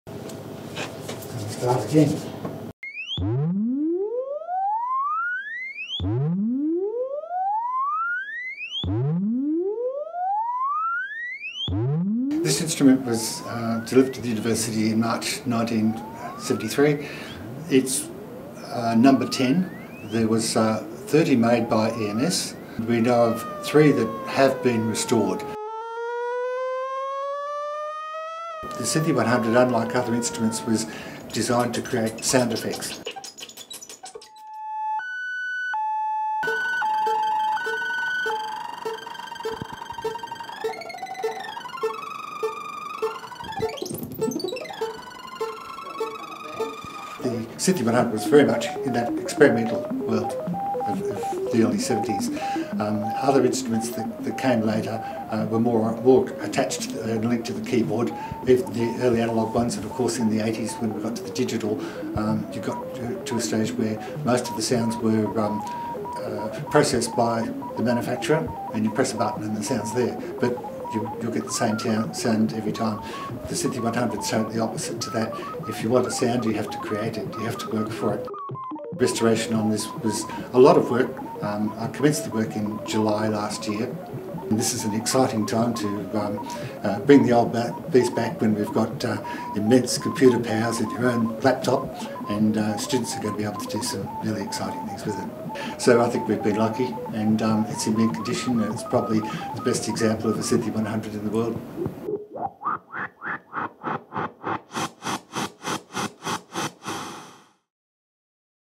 Synthi 100
Senior technician Les Craythorn reveals the fully restored Synthi 100 synthesizer, purchased by the University of Melbourne in 1973